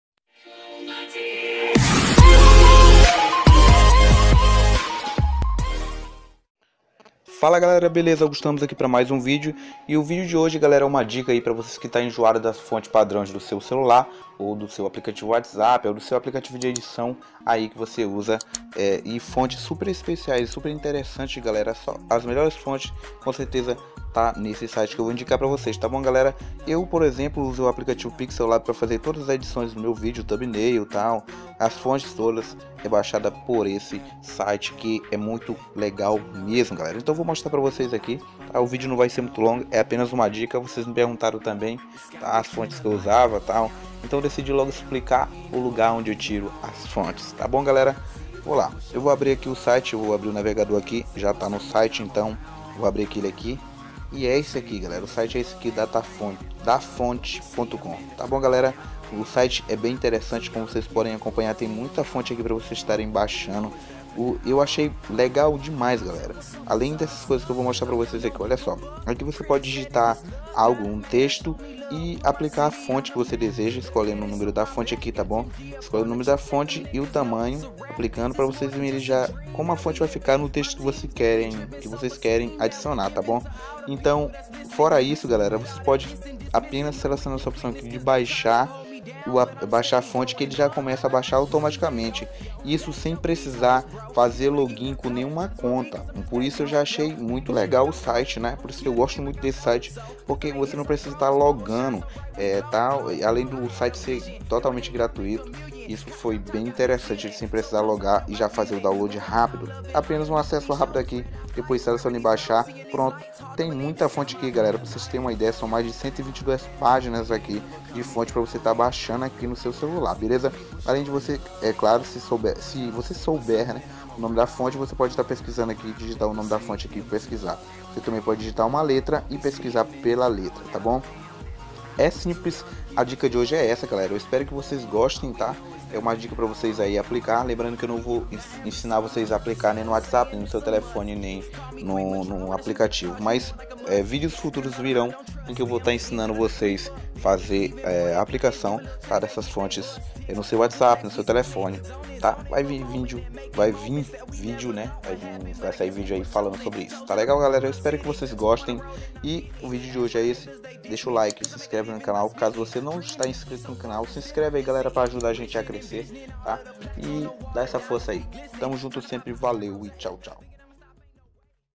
MELHOR SITE PARA BAIXAR FONTES!!! TOTALMENTE GRÁTIS!!
Fala galera beleza, aqui é Augusto Ramos pra mais um vídeo, e no vídeo de Hoje Eu Estou trazendo mais uma super dica, e a dica de hoje é um ótimo site pra vocês estarem baixando suas fontes,pra usarem em seus aplicativos,blogs etc.. Espero que gostem beleza, valeu tchau tchau!

PASSO A PASSO:
 DEPOIS DE REALIZAR O DOWNLOAD, VOCÊ PROCURA A PASTA DOWNLOAD EM SEU CARTÃO DE MEMÓRIA OU MEMÓRIA INTERNA (SE VOCÊ NÃO TEM CARTÃO DE MEMÓRIA) APÓS ENCONTRAR SUA FONTE,ELA VAIS ESTAR COMPACTADA EM ARQUIVO ZIP, VOCÊ VAI PRECISAR USAR O APLICATIVO ES FILE EXPLORER PARA EXTRAIR A FONTE QUE VOCÊ BAIXOU, APÓS EXTRAIR VOCÊ PODERÁ MOVER PARA O LOCAL DESEJADO

•lembre se Jesus é o caminho a verdade e a vida